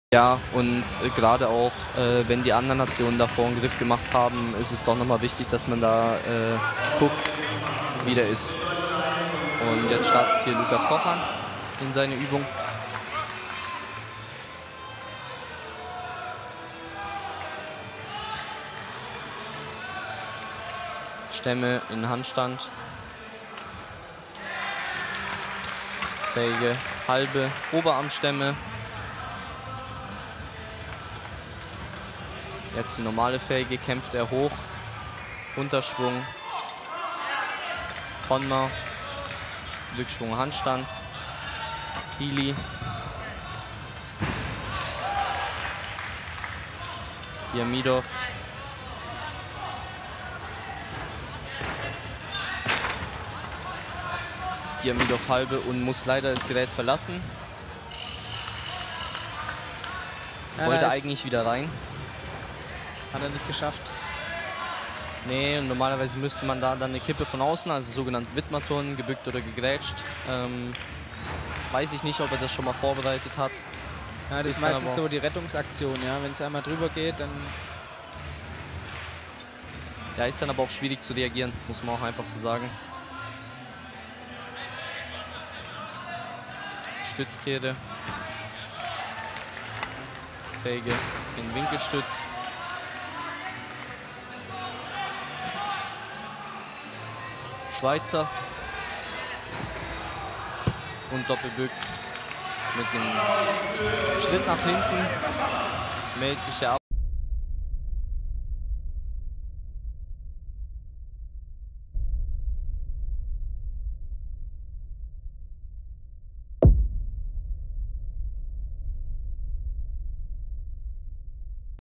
38th EnBW DTB-Pokal. Team Challenge Senior. PB KOCHAN Lucas (GER)
PB Exercise KOCHAN Lucas
D 5,5 E 6,800 Score 12,300
TEAM CHALLENGE SENIOR
DTB-Pokal 2023. Stuttgart (GER)
17/03/2023